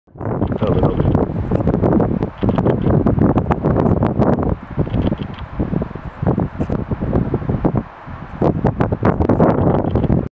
2014 05 31 1/35 Omaha beach in Westende
Prototyping the combination of light and movement at the Belgian sea side. The scale is 1/35, the bunker is produced by Italieri and the GI's are Tamiya ones.

All the magic is of course ... Magic Scale Modeling